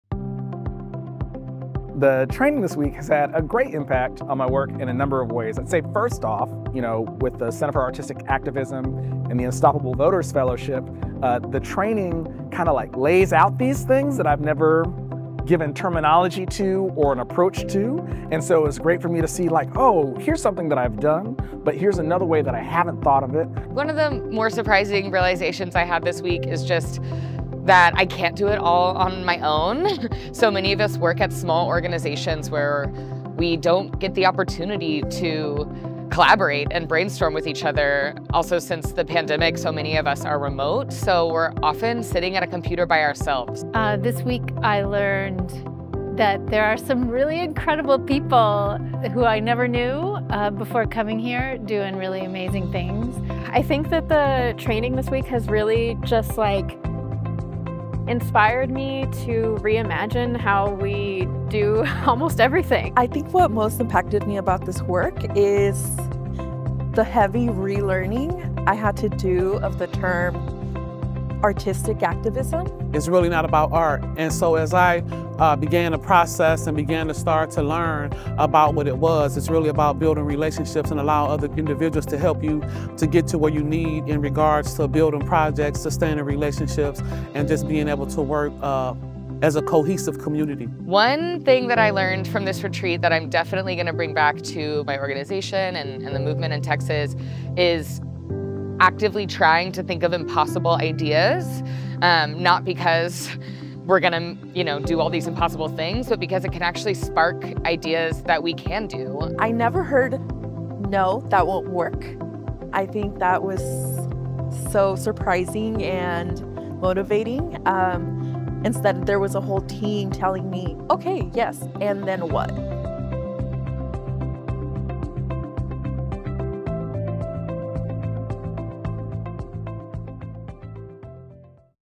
Unstoppable Voters 2023 Fellows
In 2023, we’re supporting six leading artistic activists through our Unstoppable Voters Fellowship. Learn more about these incredible voting champions and our fellow retreat that took place in Virginia Beach, VA.

Video: ReBrand LLC